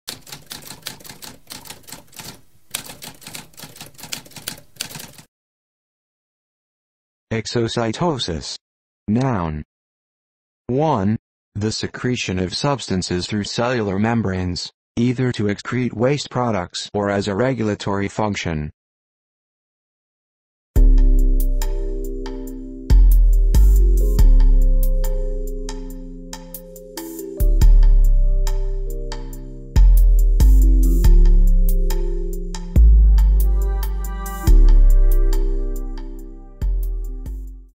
What does exocytosis mean?
A spoken definition of exocytosis.

Intro Sound:
Typewriter - Tamskp